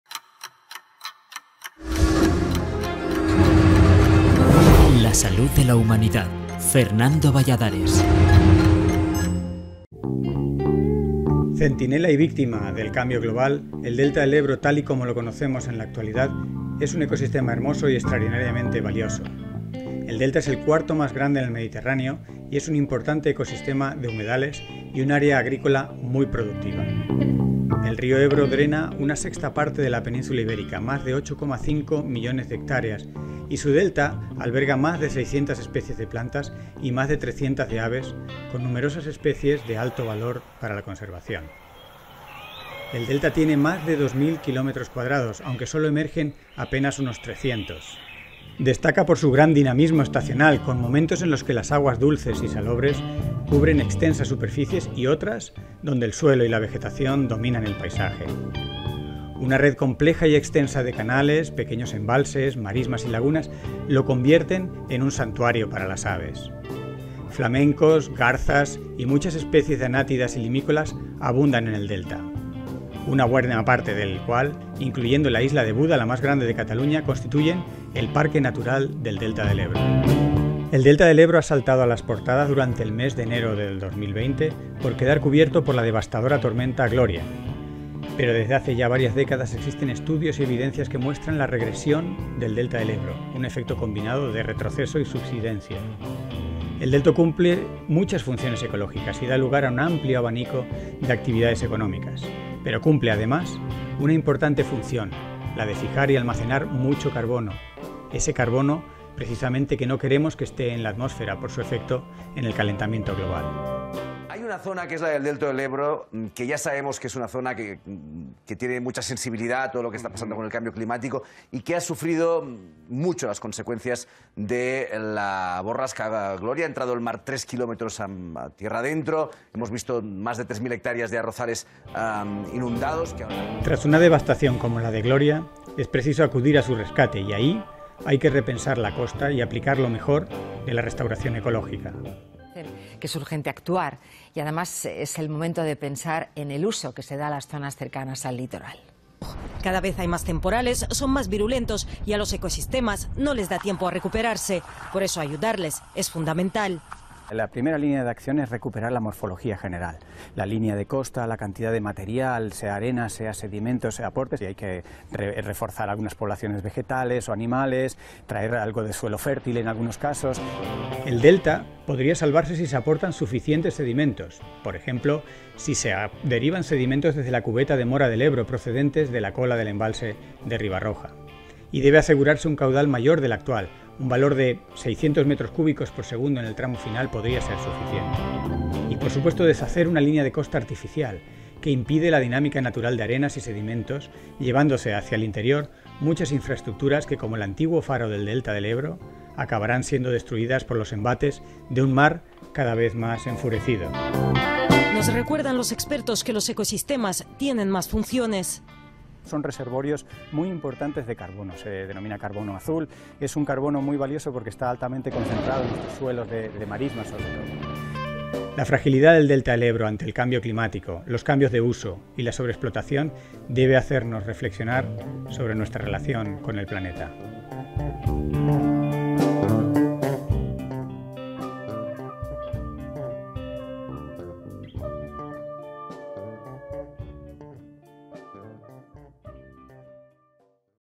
18 El delta del Ebro - Centinela y víctima del cambio global
El delta del Ebro tal y como lo conocemos en la actualidad es un ecosistema hermoso y extraordinariamente valioso. El Delta es el cuarto más grande en el Mediterráneo. Es un importante ecosistema de humedales y un área agrícola productiva. El río Ebro drena una sexta parte de la península Ibérica (más de 8,5 millones de hectáreas) y el delta  alberga más de 600 especies de plantas y más de 300 de aves con numerosas especies de alto valor de conservación. El delta Tiene más de 2000 km2 pero solo emergen por encima del nivel del mar, y por apenas unos metros, algo más de 300 km2.

Destaca por su gran dinamismo estacional, con momentos en los que las aguas dulces y salobres cubren extensas superficies y otras donde el suelo y la vegetación dominan el paisaje.

Una red compleja y extensa de canales, pequeños embalses, marismas y lagunas lo convierten en un santuario para las aves. Flamencos, garzas y muchas especies de anátidas y limícolas abundan en el delta, una buena parte del cual, incluyendo la isla de Buda, la mas grande de Cataluña, constituyen el parque natural del Delta del Ebro.

El delta del Ebro ha saltado a las portadas durante el mes de enero del 2020 por quedar cubierto por la devastadora tormenta “Gloria” pero desde hace ya varias décadas existen estudios que ponen de manifiesto la regresión del delta del Ebro –un efecto combinado de retroceso y subsidencia–.

El Delta cumple muchas funciones ecológicas y da lugar a un abanico de actividades económicas, pero cumple además una importante función, la de fijar y almacenar mucho carbono, ese carbono que no queremos que esté en la atmósfera por su efecto en el calentamiento global
Tras una devastación como la de Gloria es preciso acudir a su rescate, Restauración ecológica.
El Delta podría salvarse si se aportan suficientes sedimentos. Por ejemplo, si se derivan sedimentos desde la cubeta de Mora del Ebro procedentes de la cola del embalse de Ribarroja. Y debe asegurarse un caudal mayor del actual, un valor de 600 m3 por segundo en el tramo final sería suficiente.
Y deshacer una línea de costa artificial que impide la dinámica natural de arenas y sedimentos, llevándose hacia el interior muchas infraestructuras que como el antiguo faro del Delta del Ebro acabarán siendo destruidas por los embates de un mar cada vez ams enfurecido
La fragilidad del Delta del Ebro ante el cambio climático, los cambios de uso y la sobrexplotación debe hacernos reflexionar sobre nuestra relación con el Planeta.